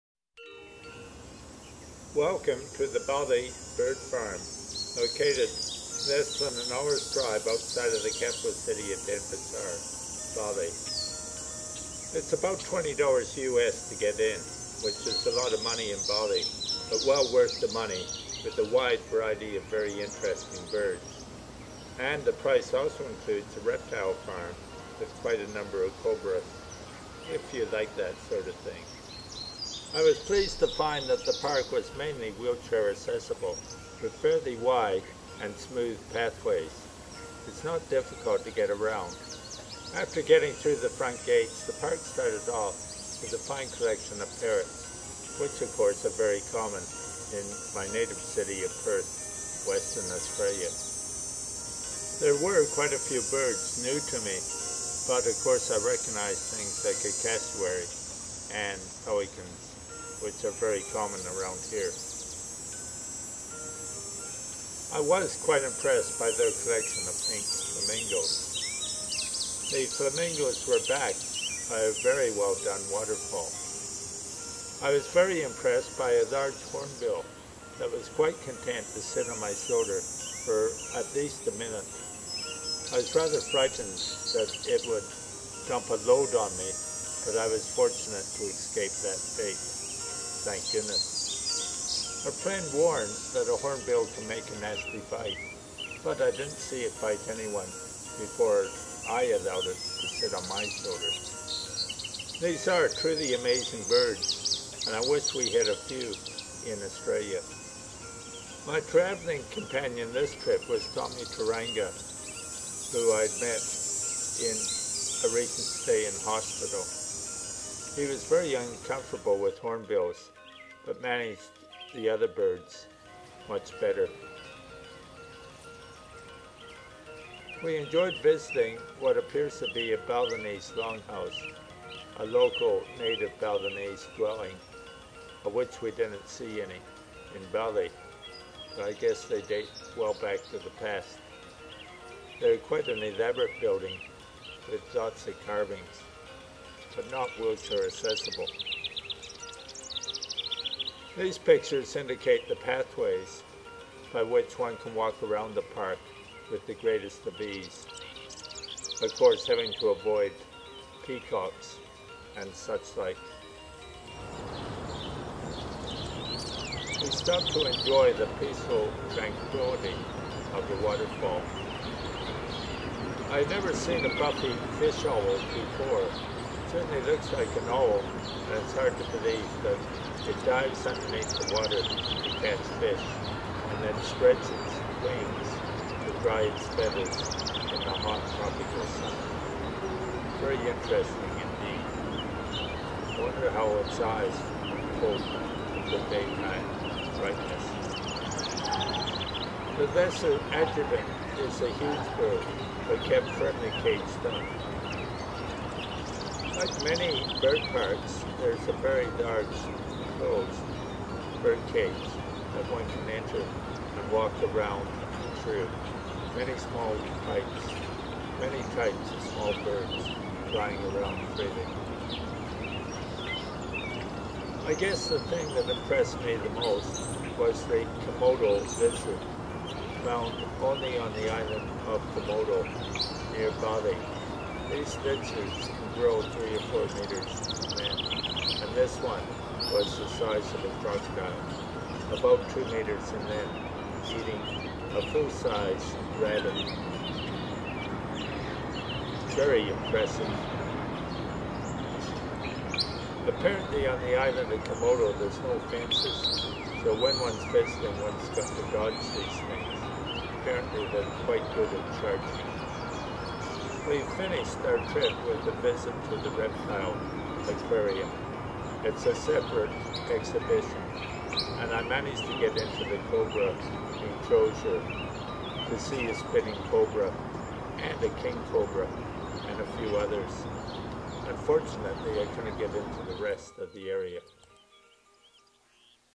Bali Bird Park BALI INDONESIA SEPT 2009
Find yourself at the Bali Bird Park to witness the largest and finest collection of Indonesian birds in the world plus fantastic birds from Africa & South America.
Encompassing two hectares of botanical landscape, the park provides sanctuary to almost 1000 birds of 250 different species.

Our innovative approach towards the display of rare and tropical birds has progressed from traditional exhibits to that of showcasing mixed species in their natural habitats & in large walk in Aviaries and free range throughout the park.

The park accommodates an amazing display of flora with more then 2000 tropical plants including 50 varieties of palms alone and attracting numerous butterflies.

Incorporating a breeding, research and veterinary facility within the complex, the park has a high success rate in the captive reproduction of exotic birds such as the Bird. of Paradise and Hornbill.
DON PUGH SEPT 2009